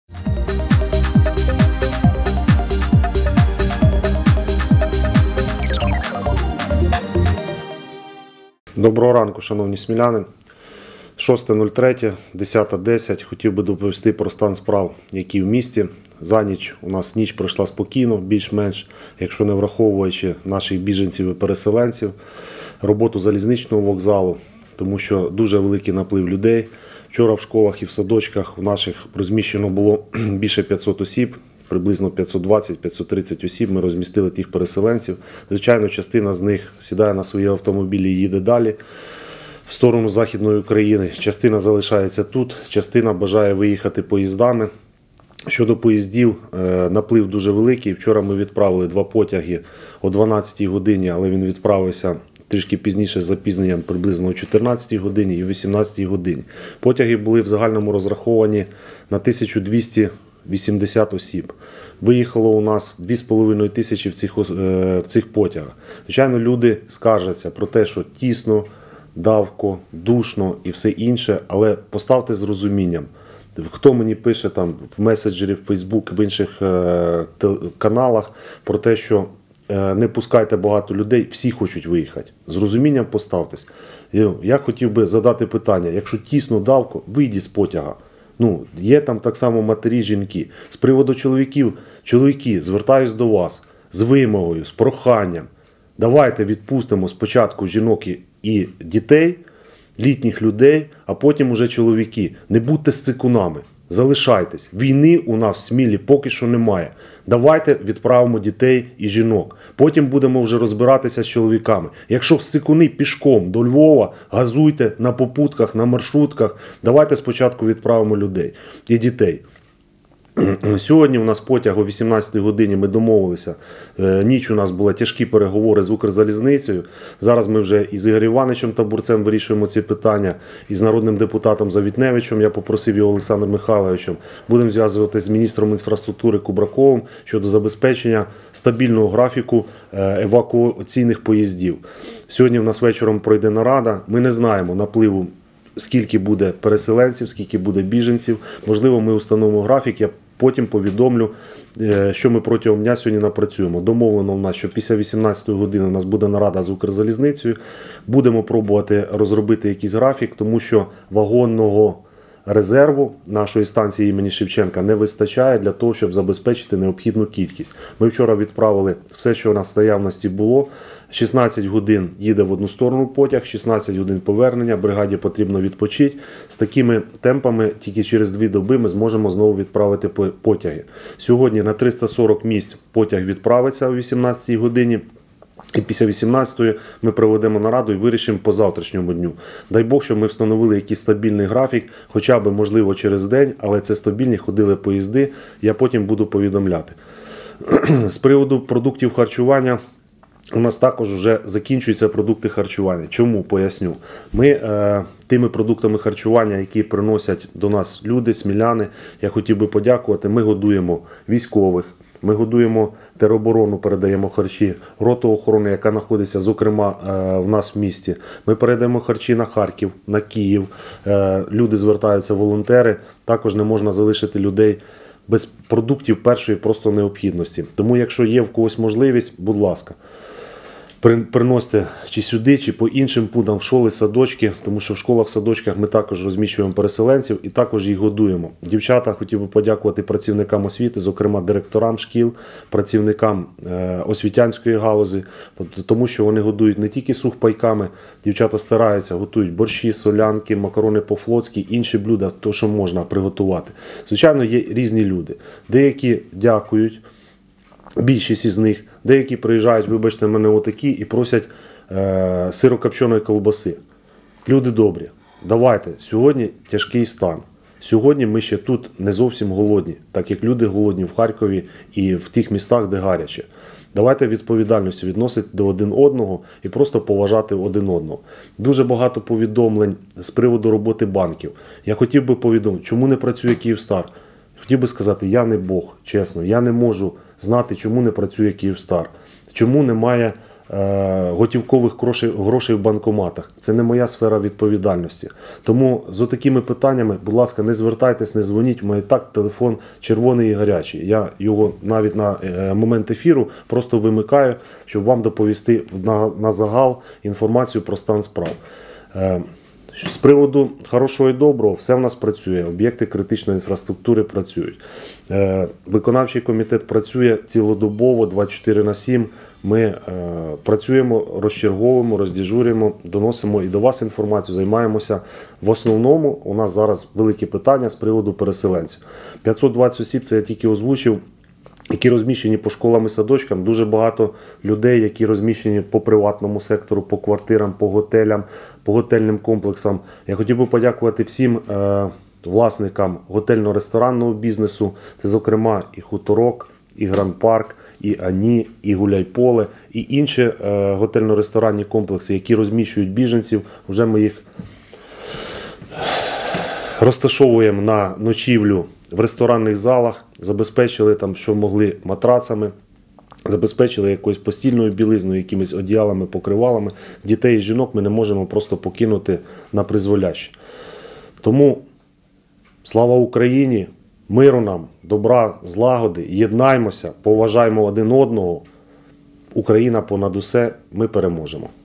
Про прийом біженців, евакуацію населення та потребу у продуктах харчування
1. 0:27 – Щовечора Сміла приймає понад півтисячі людей, котрі їдуть від голоду і руйнувань, які завдали представники «братнього» народу
2. 0:45 - Зі Сміли продовжують евакуювати людей потягами
3. 1:30 – Міський голова Сміли Сергій Ананко звертається з вимогою до чоловіків дати можливість спочатку виїхати жінкам і дітям
4. 2:00 – Евакуаційні потяги планують відправляти зі Сміли до Львова регулярно
5. 5. У Смілі потроху вичерпується продуктовий резерв. Тому бажаючих допомогти чекають на пунктах збору продуктів. Такі пункти працюють у Будинку рад (І пов.) та навчальних закладах Сміли
6. 4:40 – У Смілі працюють усі об’єкти критичної інфраструктури, виконком Смілянської міськради працює цілодобово